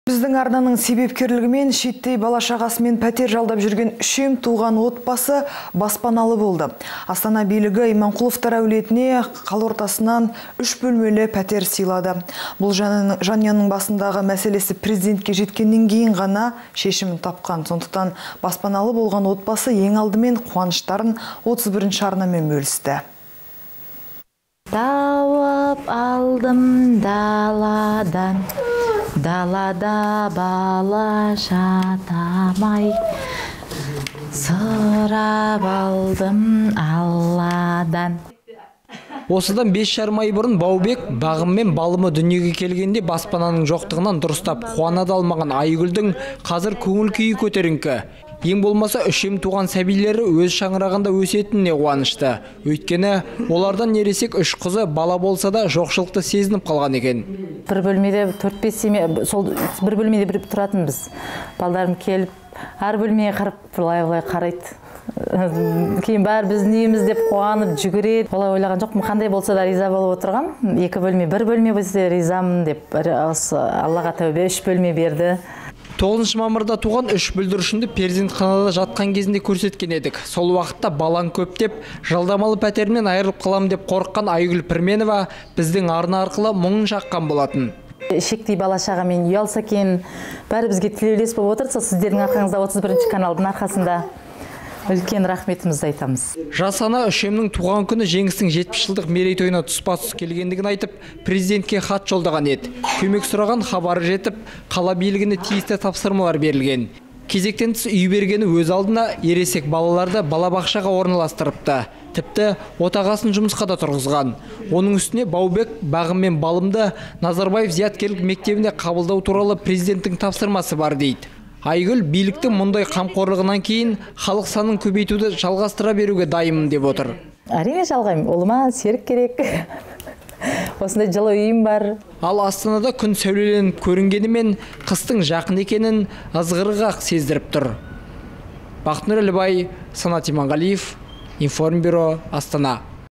Біздің арнаның себепкерлігімен үшем туған отбасы баспаналы болды
Біздің арнаның себепкерлігімен шиеттей бала-шағасымен пәтер жалдап жүрген, үшем туған отбасы баспаналы болды. Астана билігі Иманқұловтар әулетіне қала ортасынан 3 бөлмелі пәтер сыйлады. Бұл жанұяның басындағы мәселесі президентке жеткеннен кейін ғана шешімін тапқан. Сондықтан баспаналы болған отбасы ең алдымен қуаныштарын 31 арнамен бөлісті.